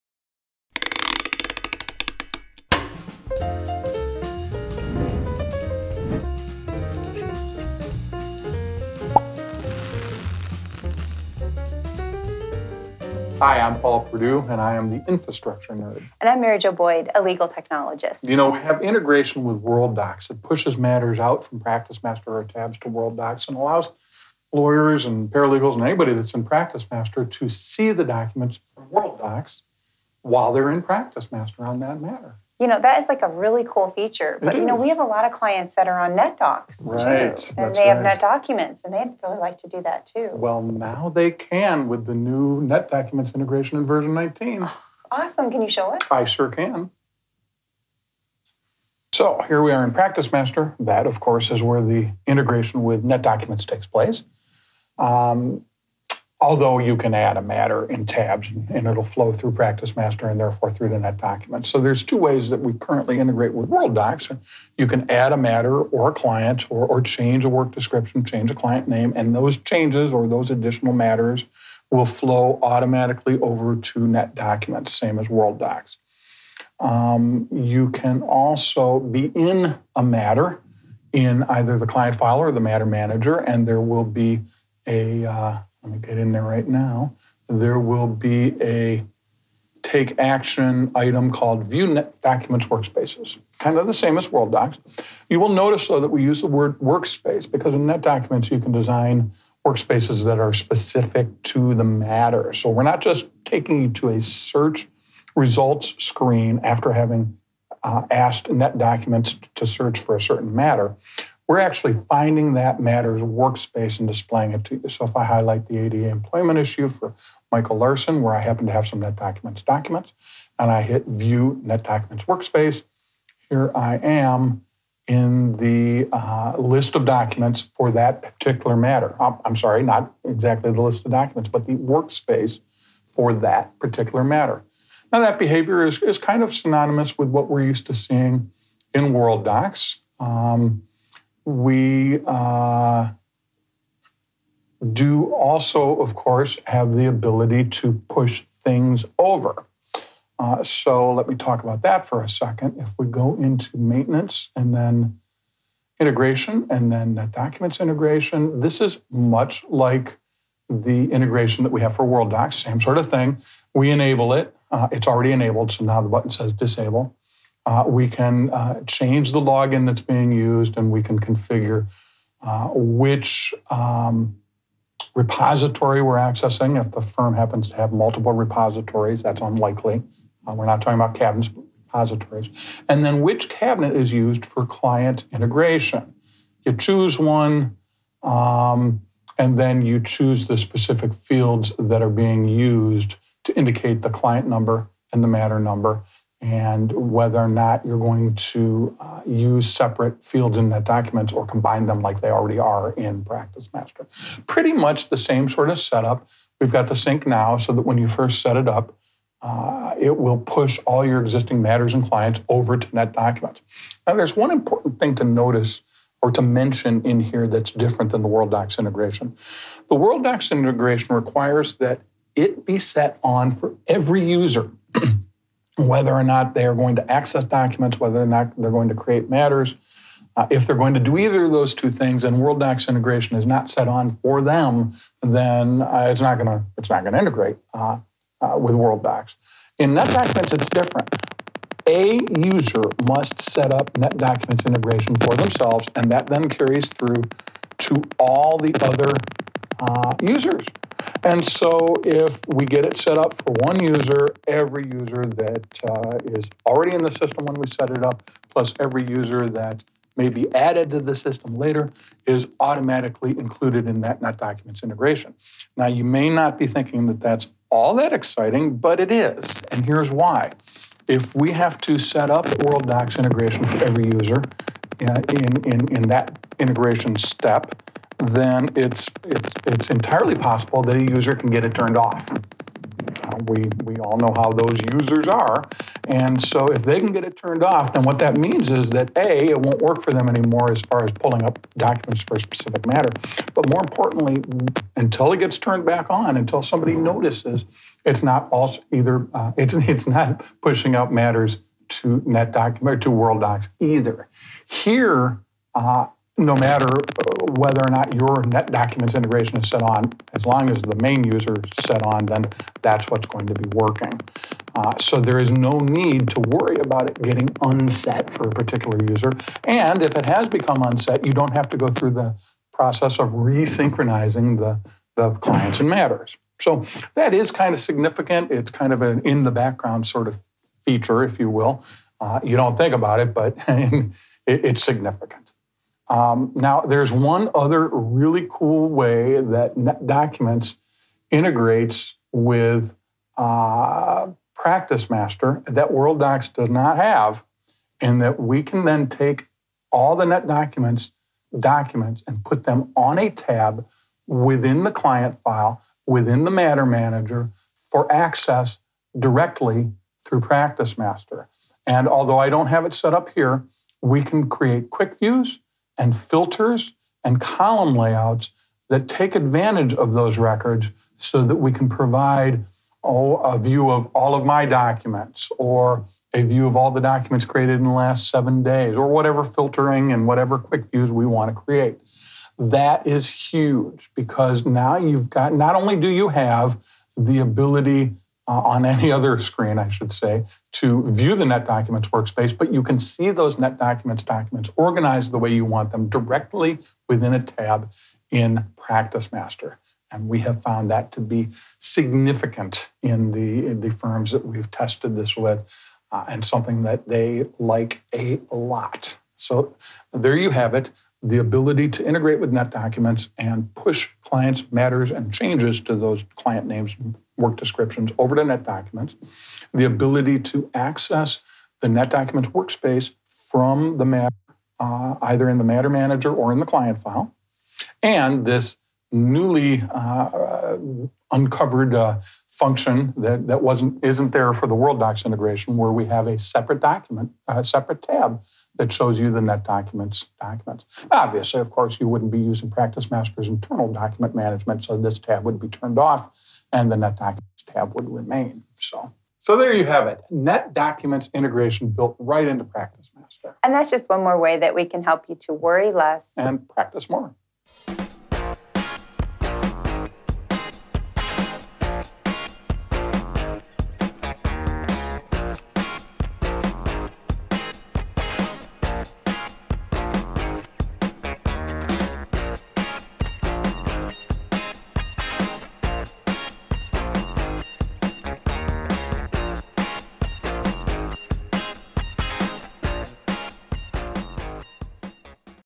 Learn how to integrate Tabs3 and PracticeMaster with NetDocuments
Join Paul as he demonstrates how to integrate Tabs3 and PracticeMaster with NetDocuments